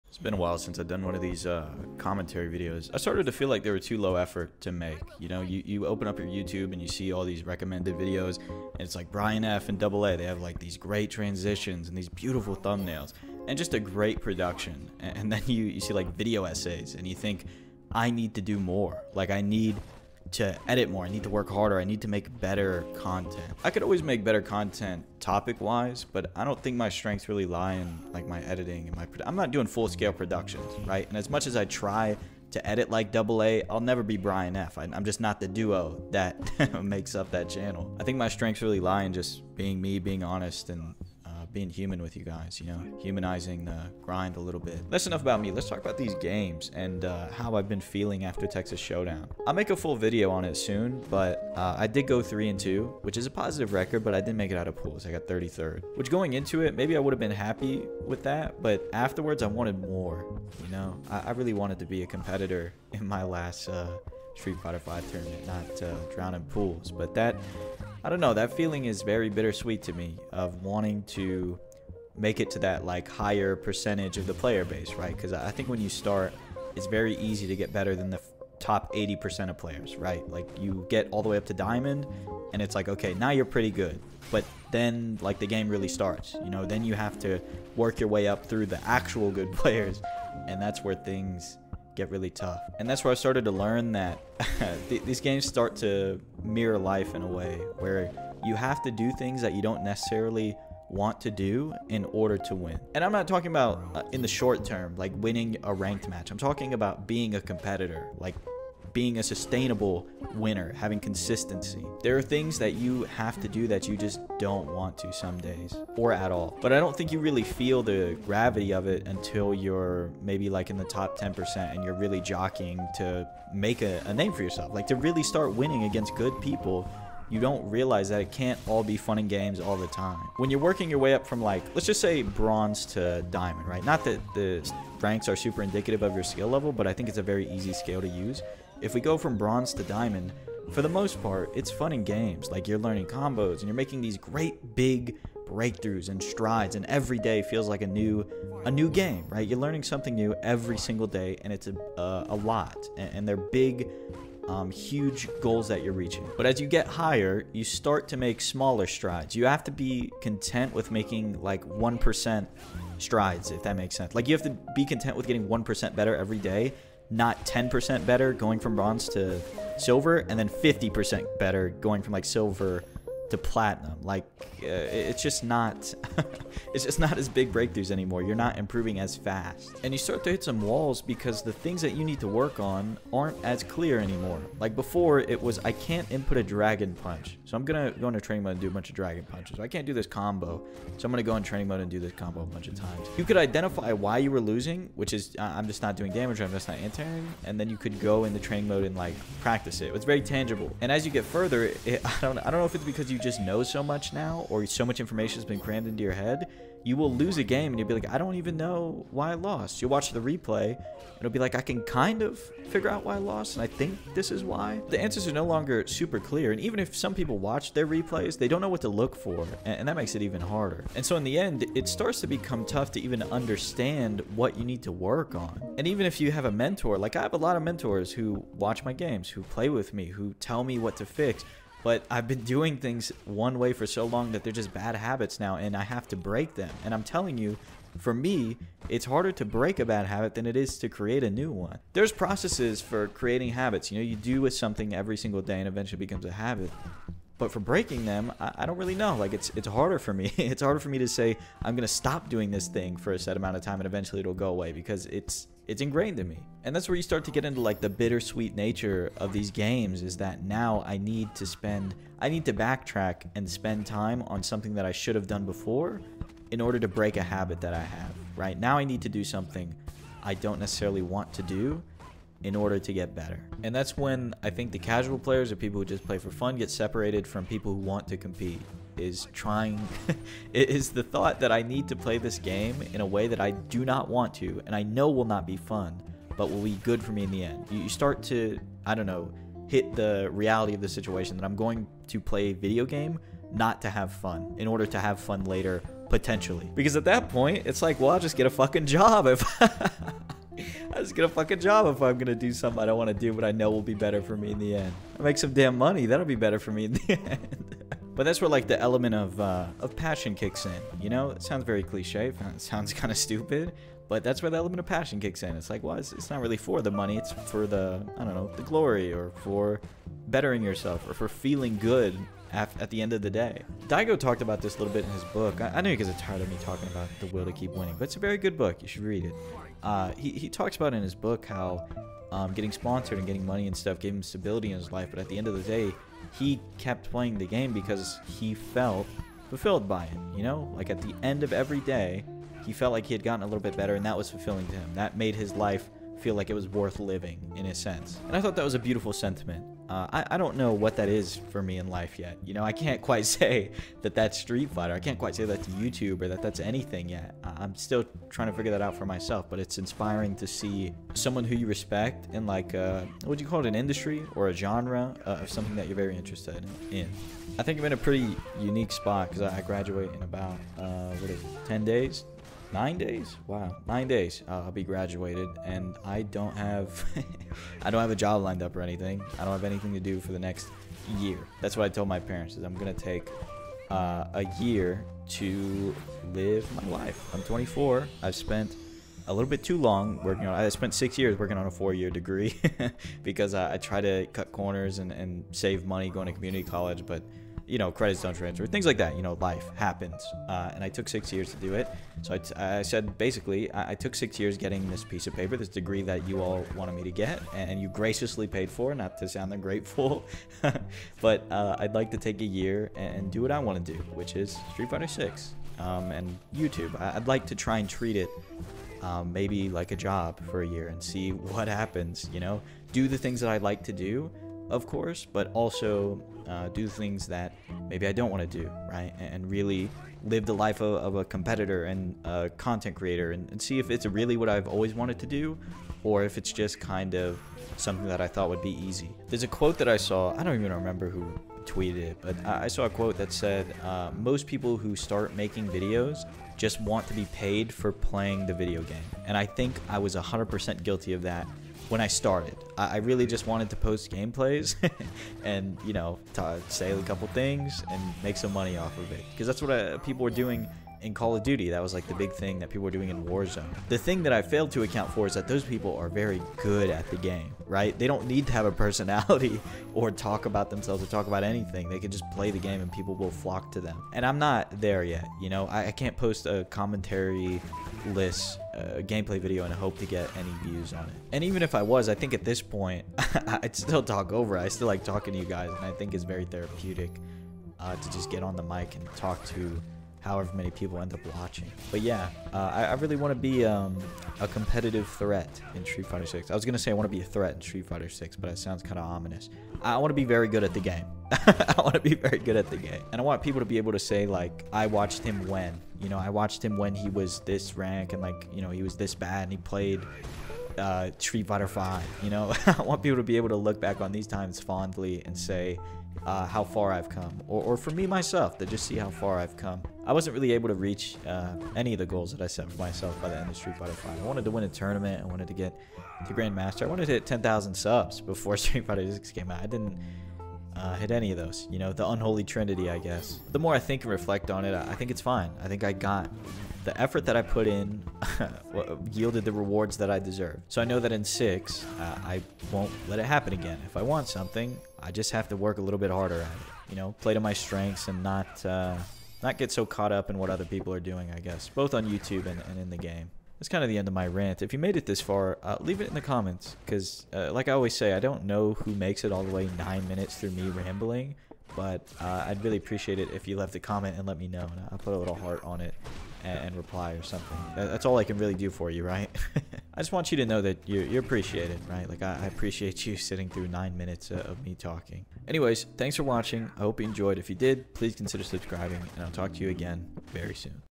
The Bittersweet Nature of Fighting Games
Today I wanted to talk about YouTube, my strengths, but most of all the bittersweet nature of fighting games and improving at them.